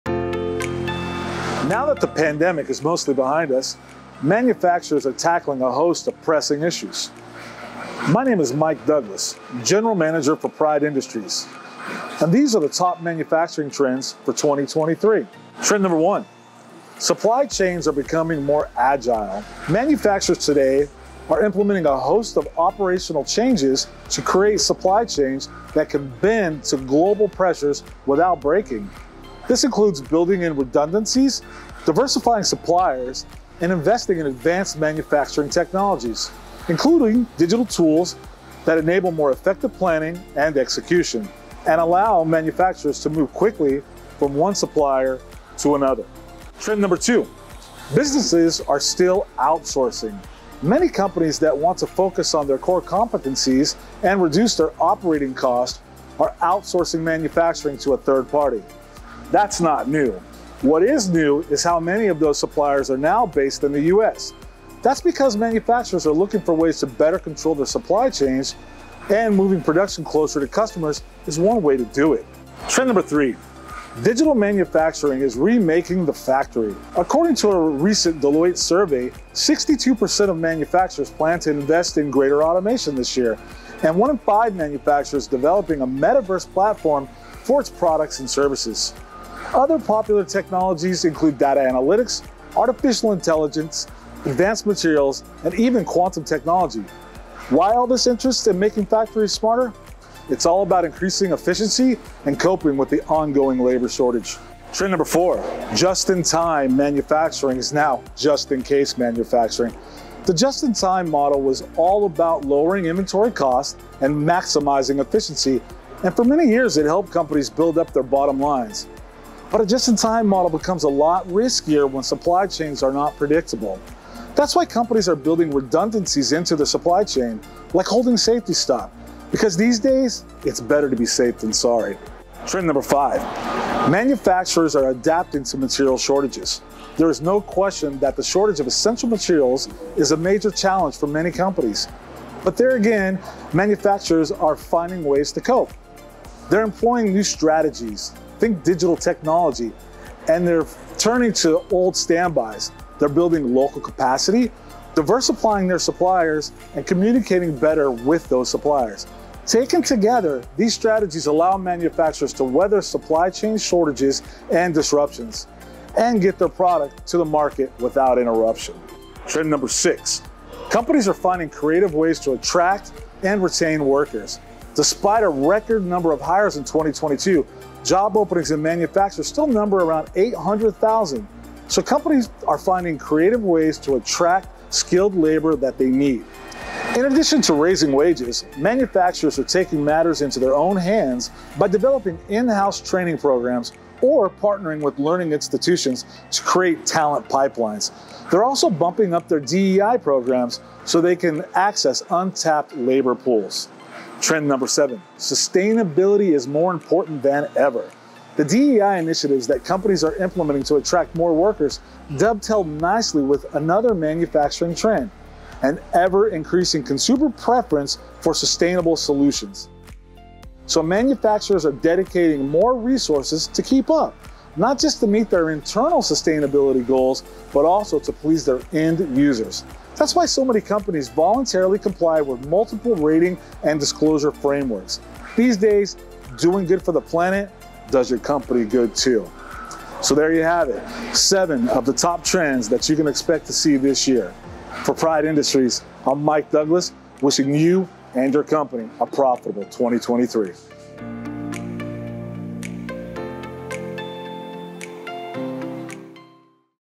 Seven Top Trends in Electronics Manufacturing 2023
Want to know where electronics manufacturing is heading in 2023? What are manufacturers looking at to get ahead and rebound from some of the difficult headwinds that have hit many companies over the last few years? In this video, we address these questions with information from industry leaders on what is trending in electronics manufacturing.
There’s a lot going on but Mike Douglas, EMS General Manager at PRIDE Industries, breaks it down for us. As an expert in the field, and with insights gathered from other top professionals, Mike has compiled seven top trends to share with you.
In this video, we find out what developments electronic manufacturers are keeping an eye on, and the changes they are implementing.
Across the manufacturing industry, as with other industries, technology is making its mark—so where is technology making the most impact in electronics manufacturing? Find out how manufacturing software and automation in manufacturing are affecting the future of factories and production workflows.
And it’s not just technology; Mike also discusses supply chains, labor shortages, DEI and sustainability, and just-in-time or just-in-case strategies. These are all future-facing aspects that look to influence electronic manufacturing in 2023. Join us to learn more.